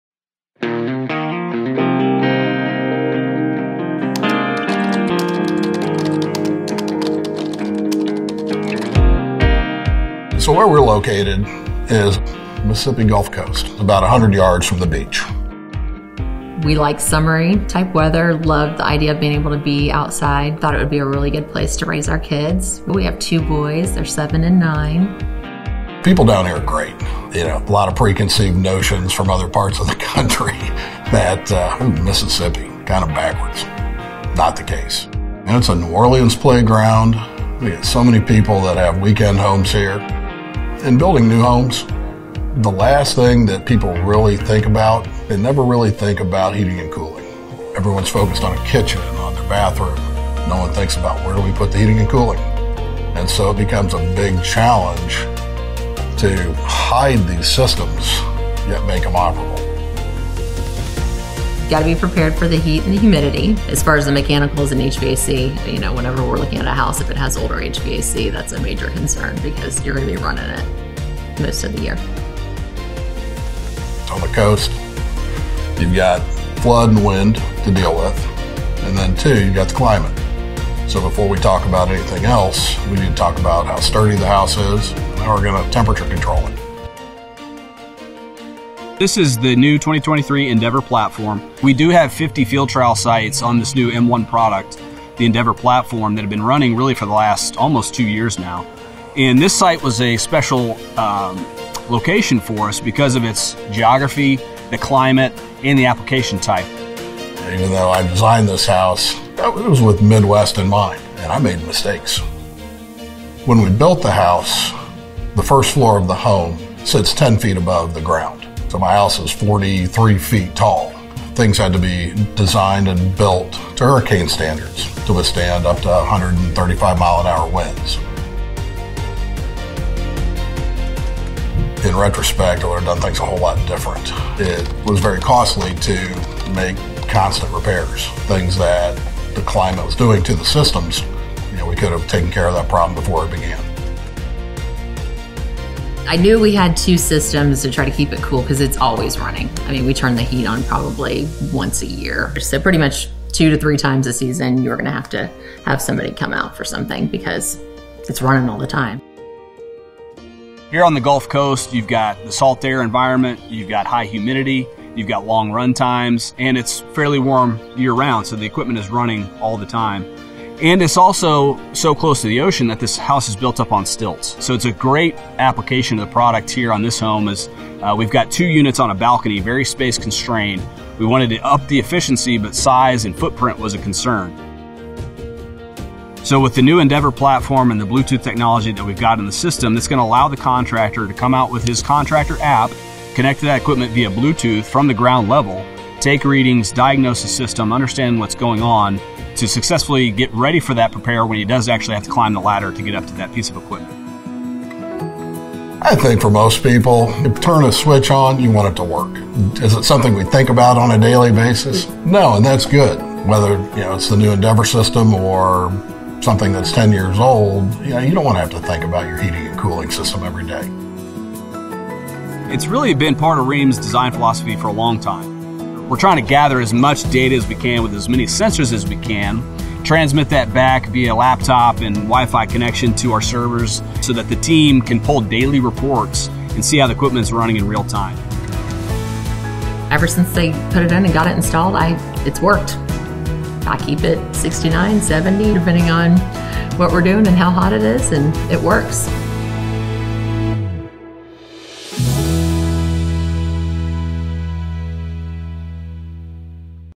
Rheem® Endeavor® Line Case Study
The new Rheem® Endeavor® Platform has been running in 50 field trial sites for the past two years to ensure that it installs and performs up to standard.

This video is the result of an important trial site on the Mississippi Gulf Coast, installed in July 2021. This study not only highlights the durability and high efficiencies of the Endeavor RP16AZ Heat Pump and RH3VZ Air Handler in a hot and humid environment where they’re running almost constantly but the importance of their small footprints and Bluetooth® connectivity, as they needed to be installed on a space-constrained balcony.

Bluetooth technology, available in many products in the Endeavor Line, allows contractors to come in, use their contractor app and connect to the Endeavor equipment via Bluetooth from the ground level. This makes it easy take readings, diagnose the system and understand what’s happening to successfully prepare for a repair before climbing a ladder or entering a tight space to access the equipment.

Visit Rheem.com/HVACKnowZone to learn more about the upcoming regulation changes and how Rheem continues to rise to the challenge.